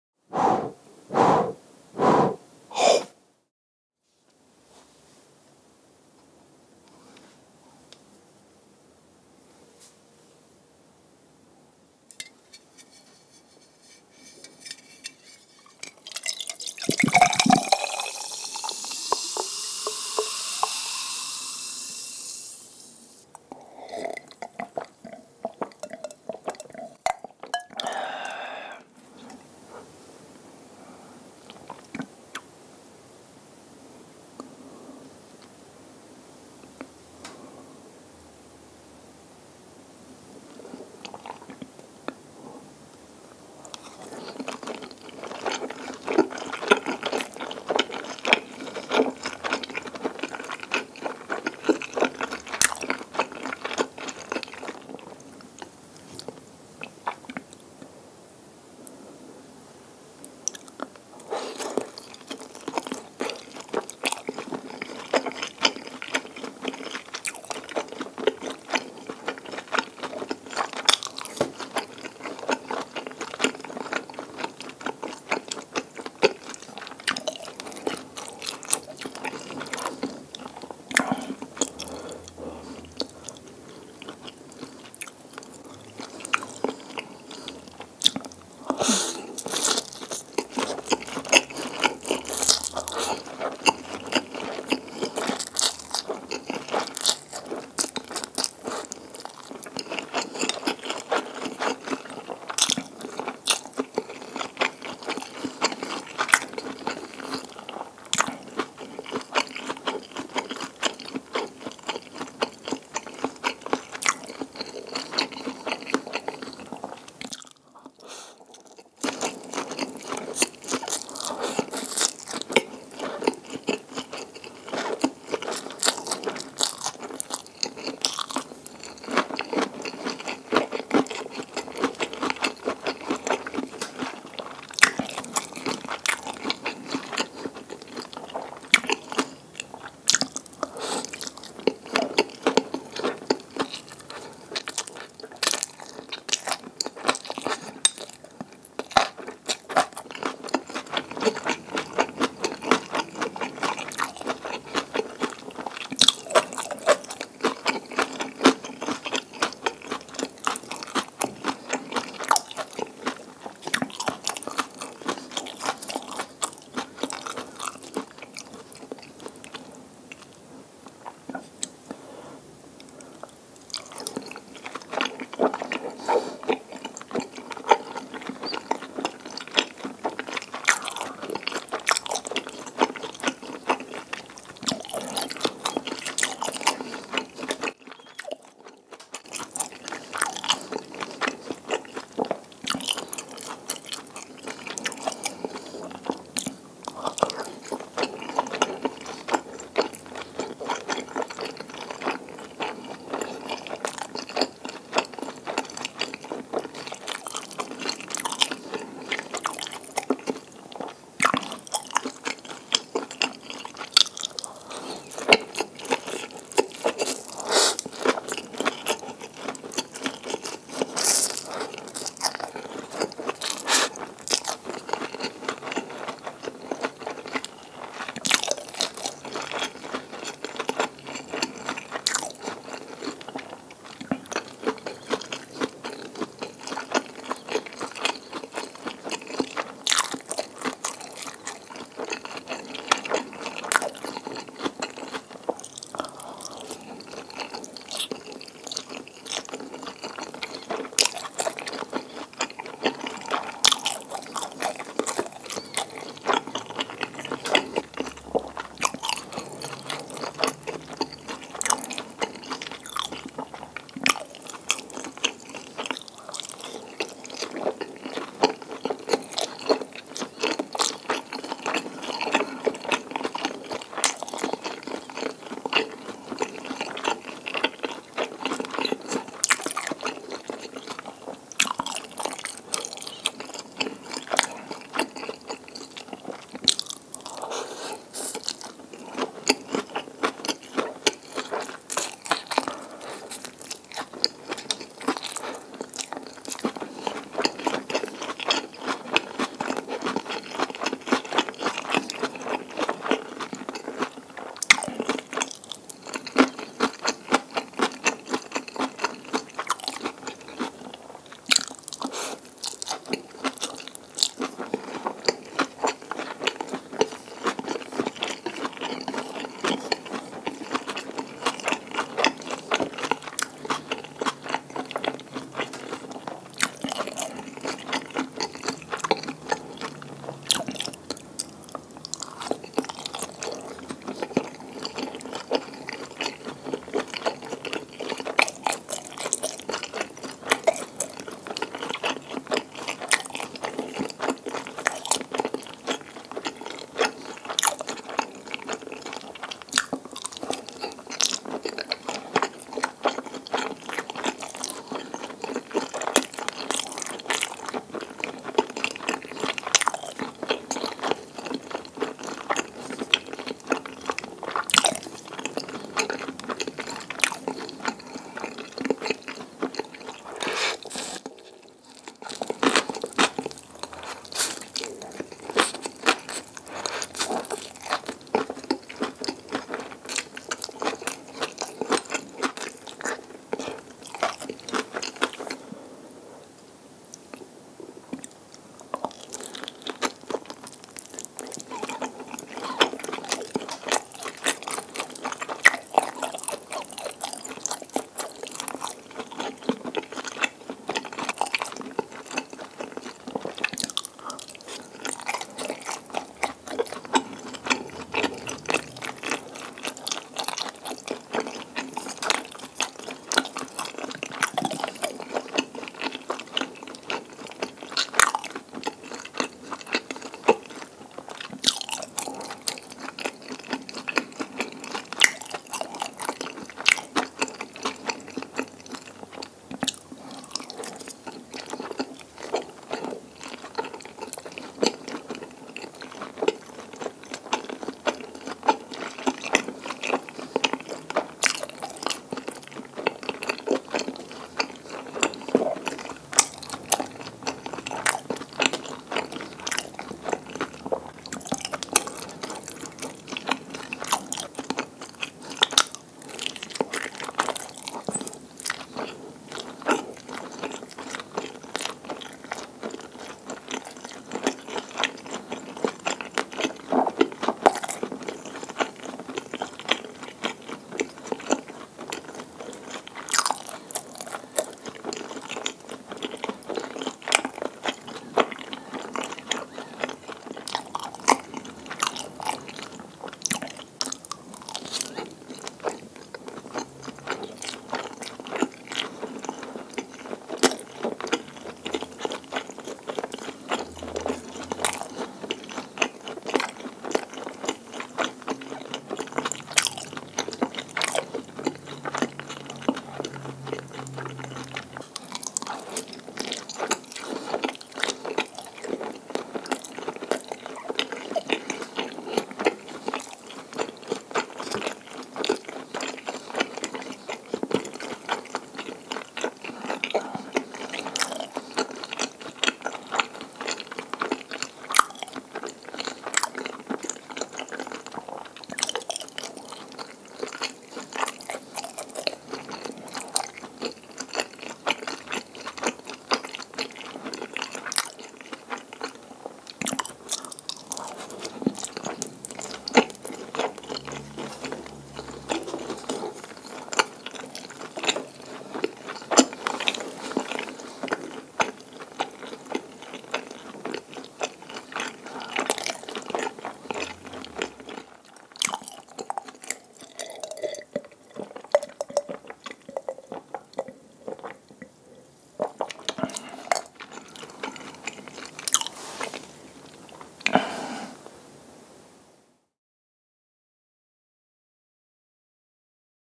ASMR EATING CHICKEN MOMOS & SPICY VEG CHOWMEIN MUKBANG (No Talking) EATING SOUNDS | BUN ASMR
//GEARS USED:
Backup Microphone
SD Card
Studio Lights